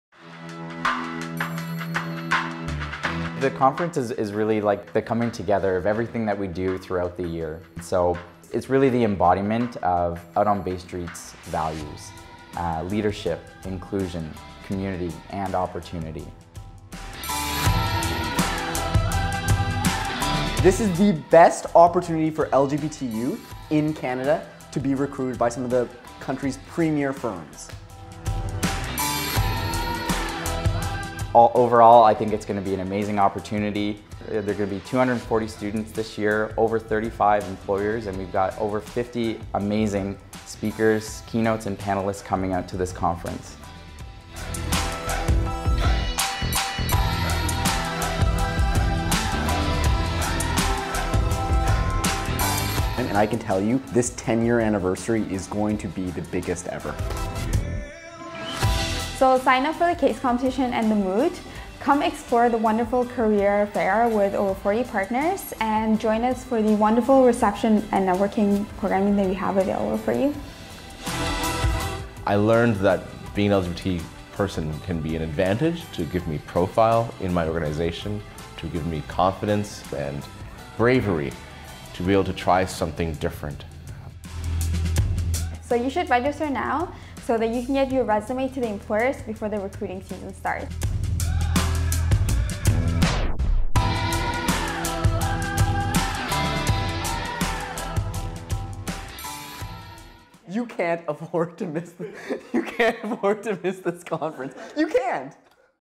Out On Bay Street Annual Conference 2016 Trailer
Join us on Sept. 16 & 17 in Toronto for the 2016 Out On Bay Street Annual Conference.

Out On Bay Street is committed to connecting the LGBTQA leaders of tomorrow with successful futures today.